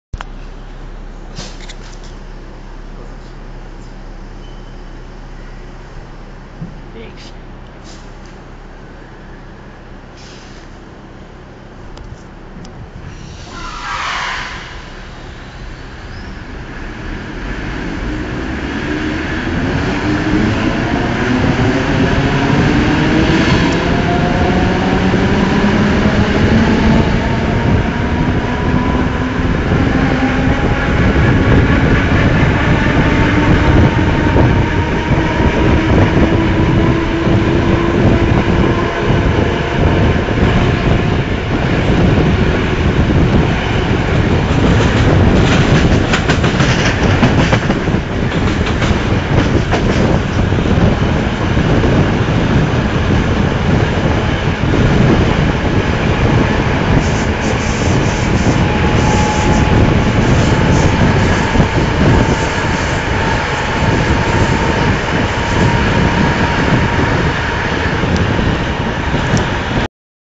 Olympische VIRM vanuit een Mat '64 Plan-V
Vertrekkend vanuit station Den Haag HS in de Stoptrein naar Dordrecht komen we de Olympische VIRM tegen.

Veel kijkplezier & Bedankt voor het kijken.